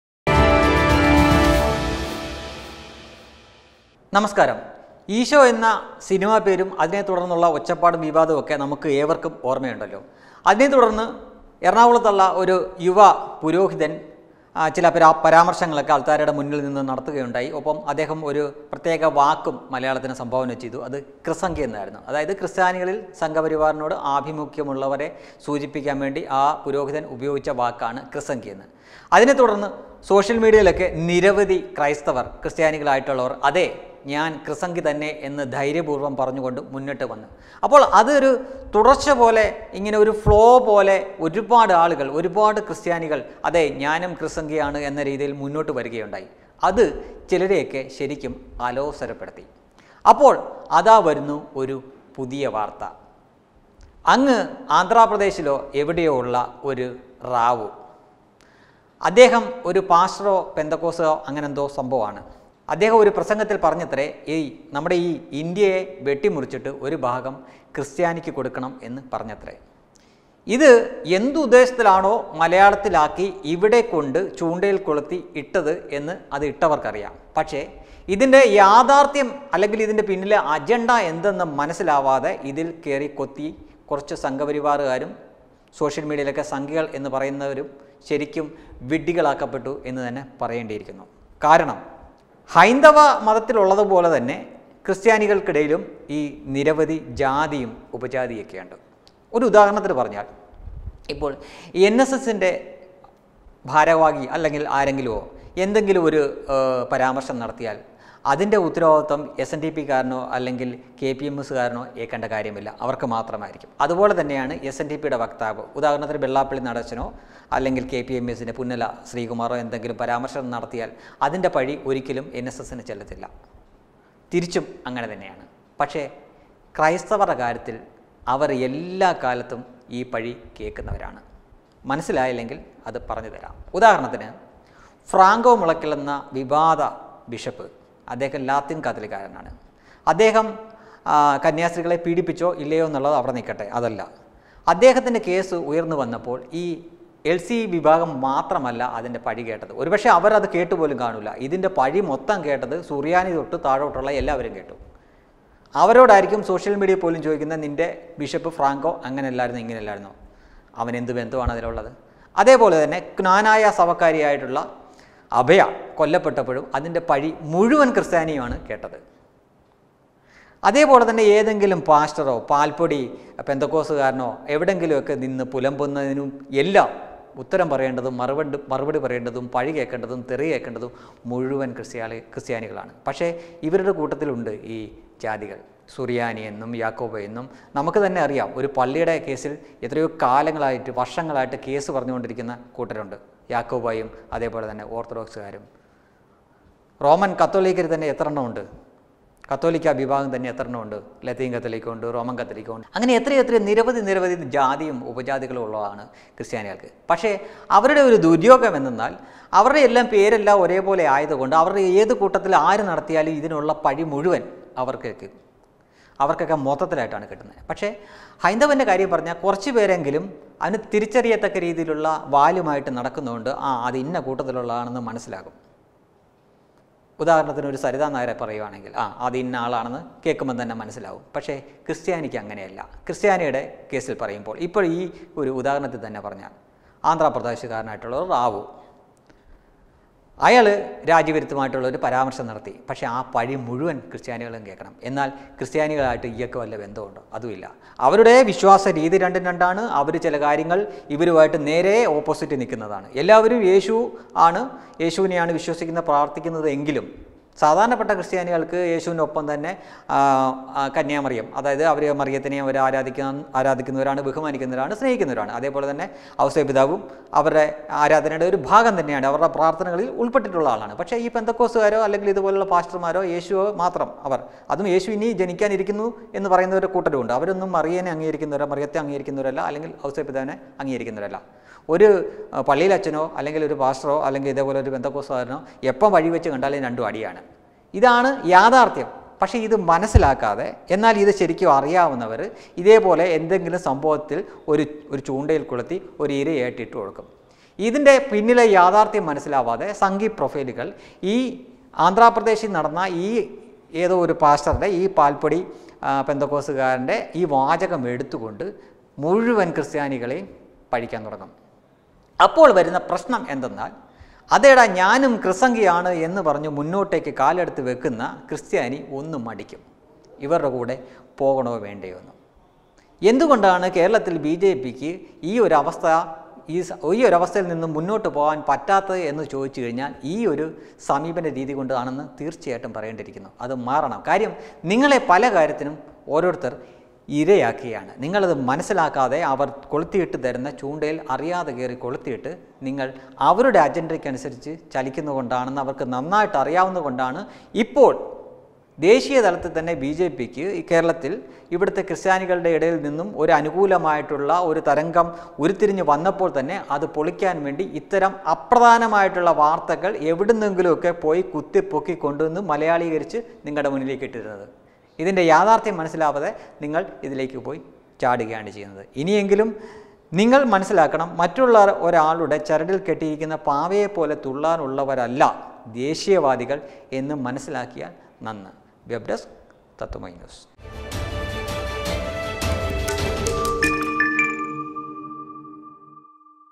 ഹിന്ദുവും ക്രിസ്ത്യനും ഒരിക്കലും ഒന്നിക്കരുത്, അതിനാണ് ഈ കുത്തിത്തിരിപ്പെല്ലാം | KRISANGI
ക്രിസംഘികളുടെ എണ്ണം പെരുകുന്നു , ഒപ്പം കുത്തിത്തിരുപ്പുകളും.| KRISANGI